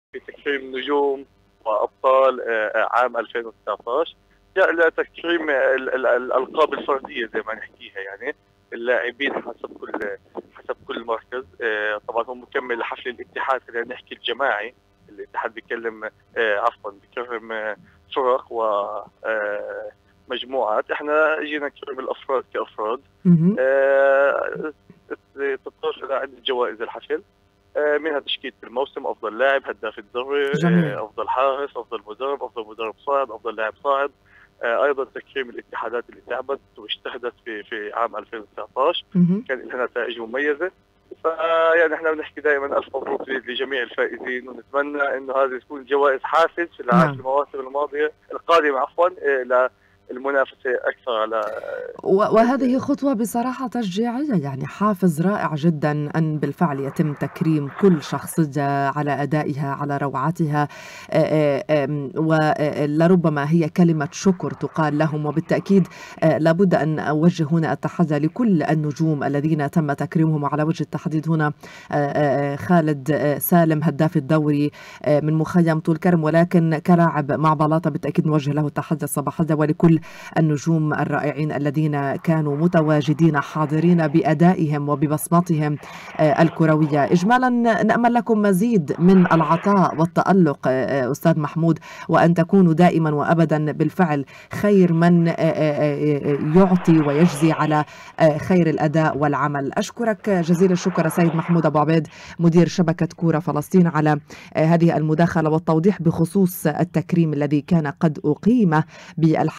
شبكة كورة تكرم نجوم الكرة الفلسطينية ....
تلفزيون الفجر | شبكة كورة تكرم نجوم الكرة الفلسطينية
المزيد مع محمود ابو عبيد مدير شبكة كورة فلسطين